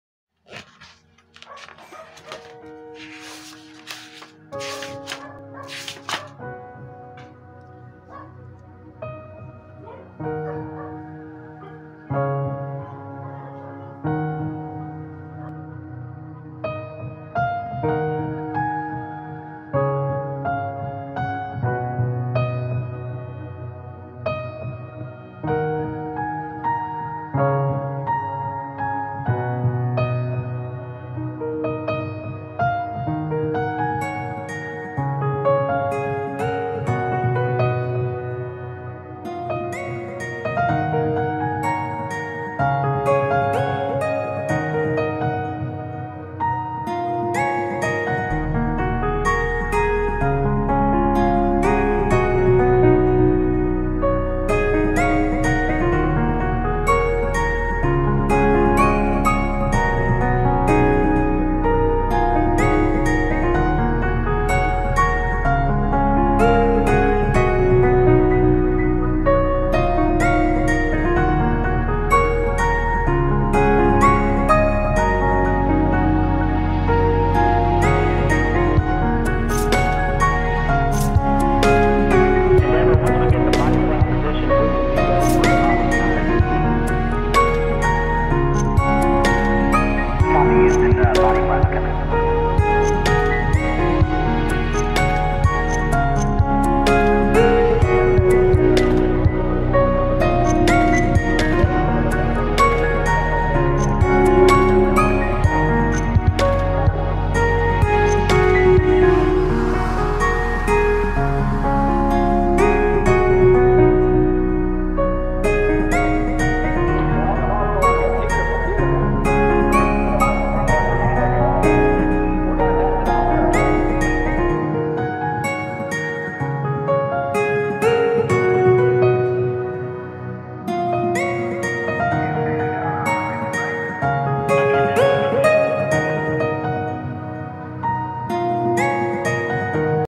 Ibis Paint X speedpaint | "Stuck in The Hourglass"
I did a little speedpaint for a piece I had to make for school. It's an expression of my current identity and mental state in the middle of quarantine as of now.